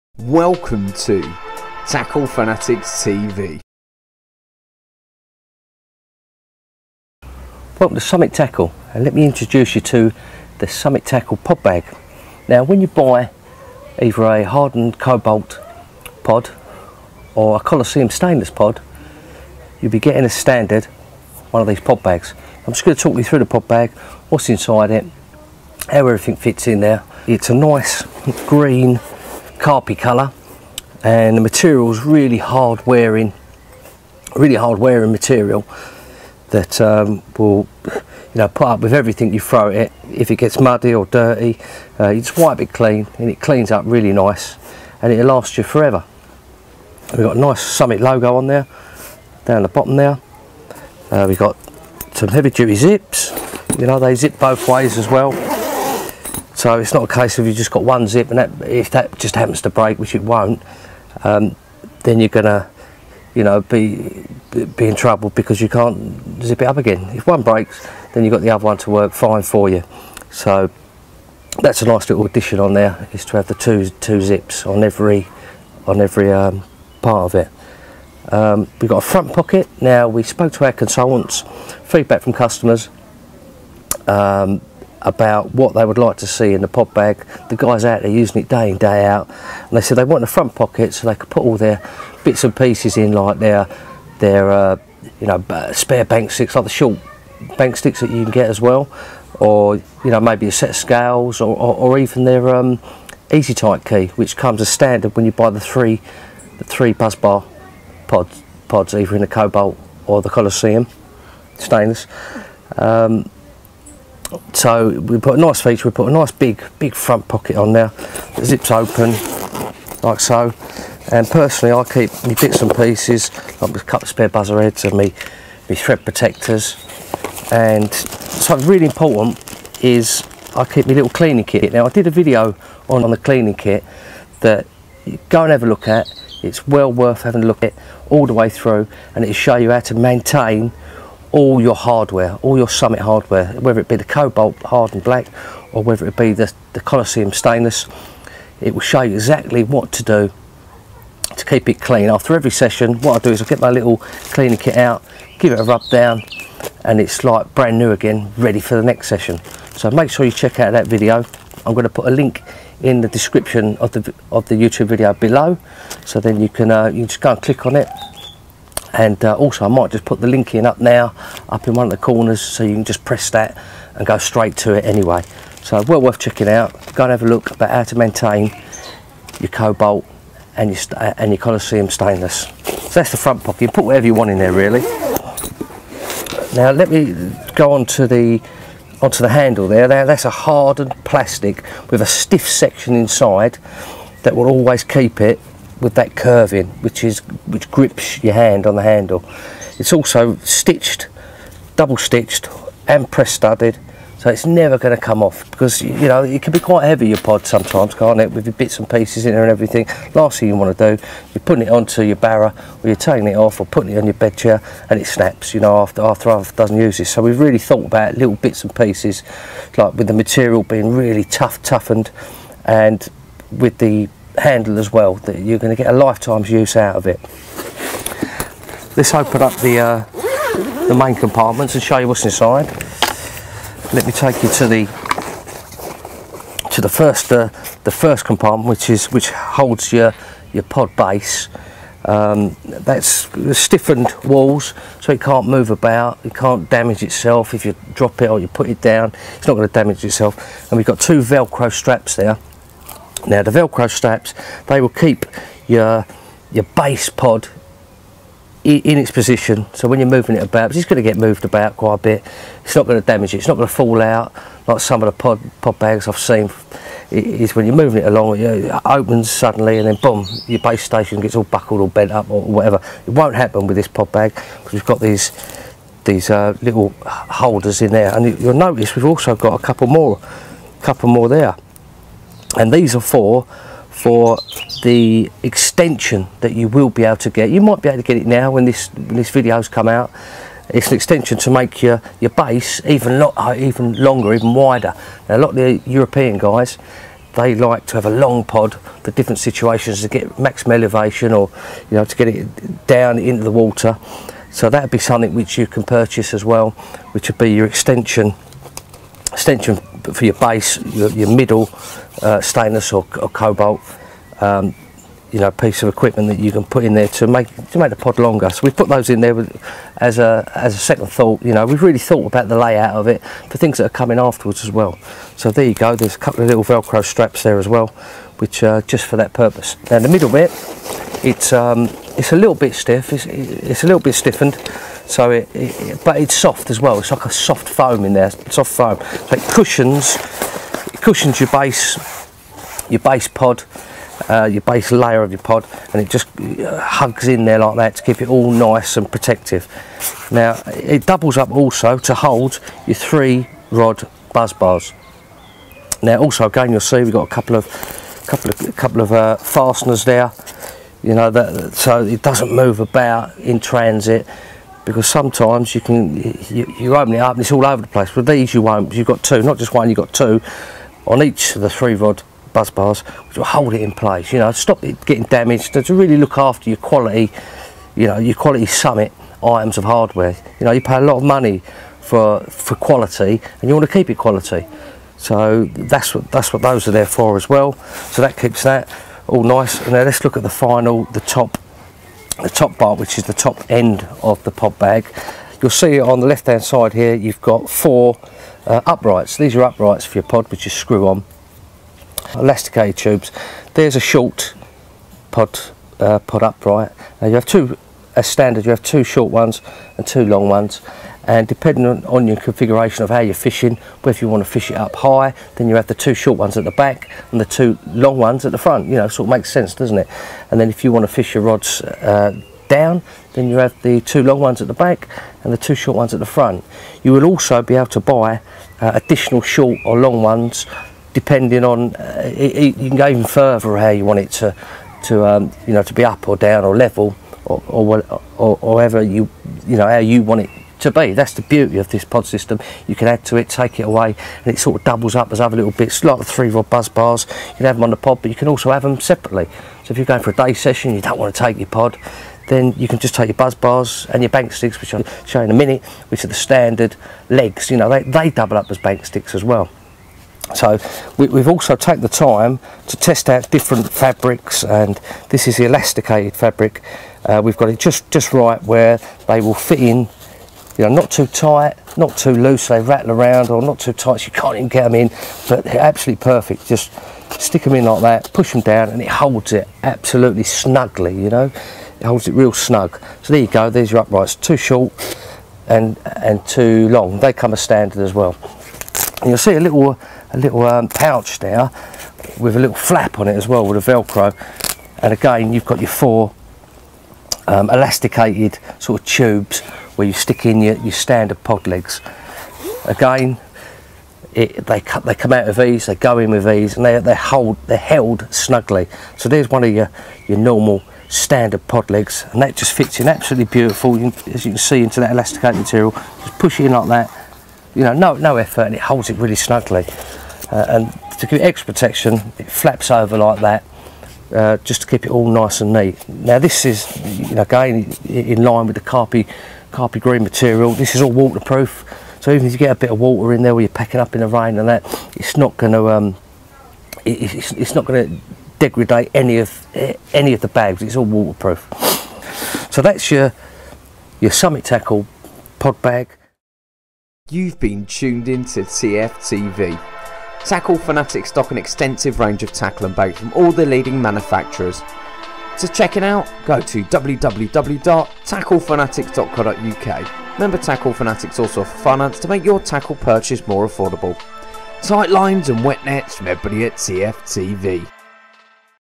Tackle Fanatics TV - Summit Tackle Pod Bag
Leon Bartropp takes a look at Summit Tackle's Pod Bag.

Tight lines, TF TV